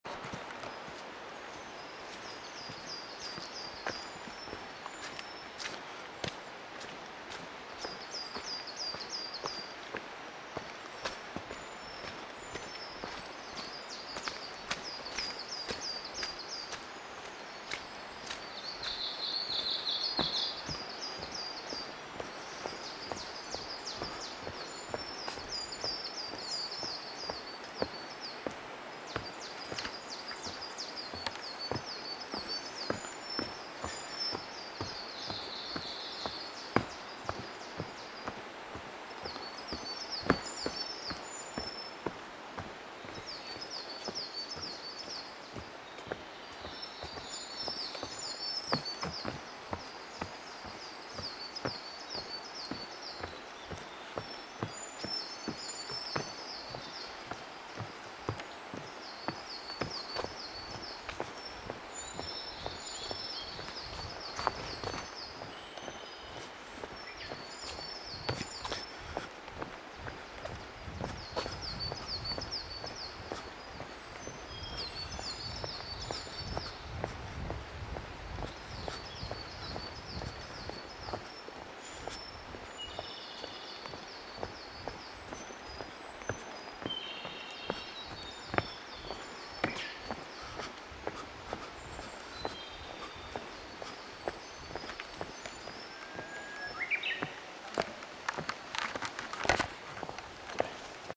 Hiking in Oze
This area is between Oze-numa(Oze pound) and Oze-ga-hara(Oze wetlands)
Recorded　on 18 June 2011 in morning.
Oze located north to Tokyo.There are many highland plants and birds in woods.